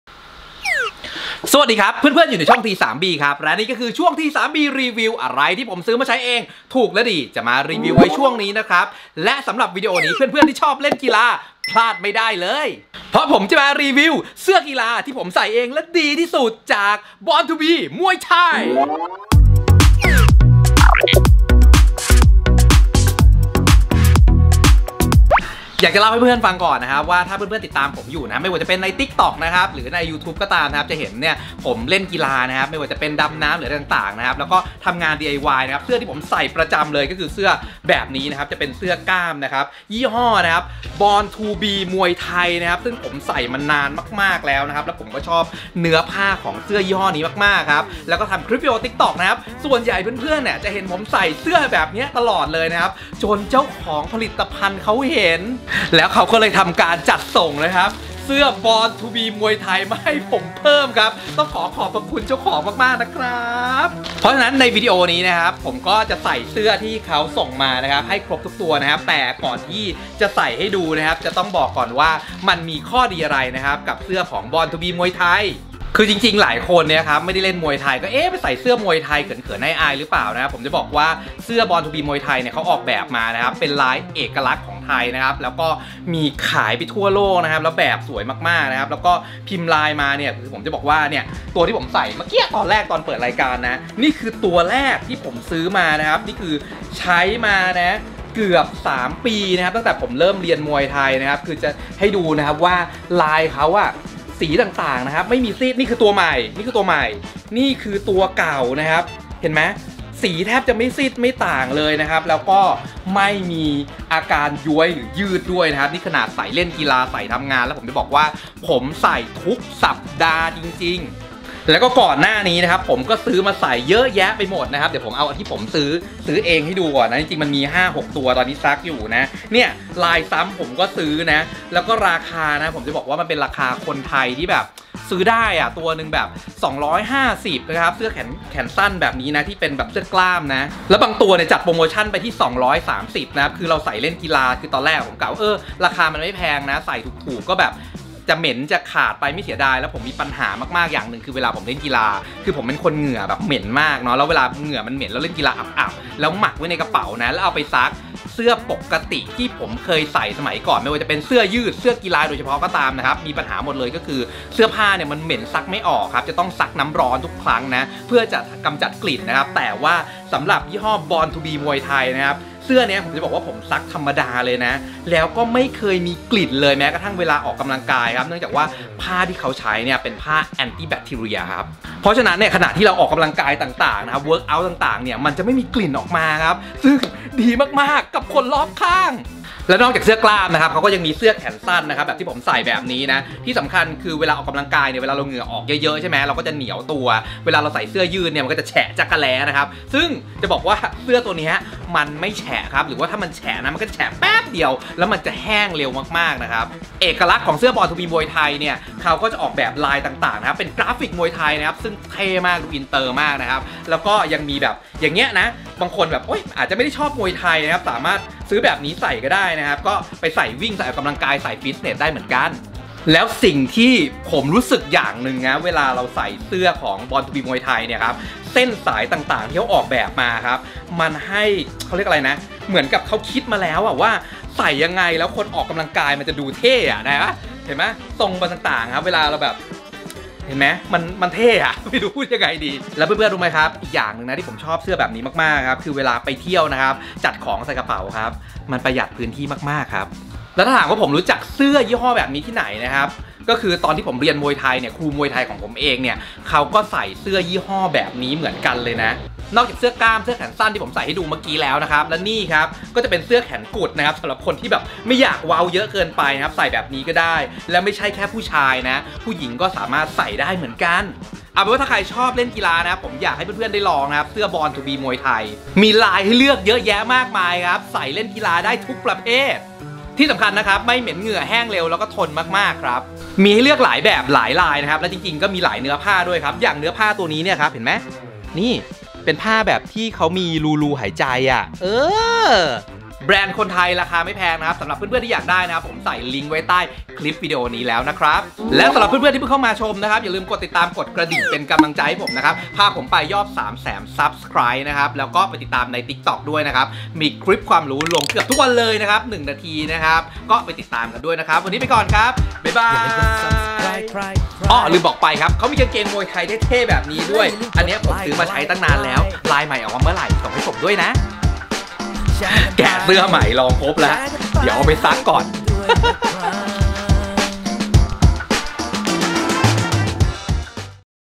รีวิวเสื้อที่ผมชอบมากที่สุด Born To Be Muay Thai l T3B
ผมใส่ทุกวันจริงๆเป็นเสื้อที่ใส่สบายมาก ไม่เหม็น ไม่คัน ไม่ร้อน ลองซื้อไปใส่ดูครับแล้วจะติดใจจริงๆ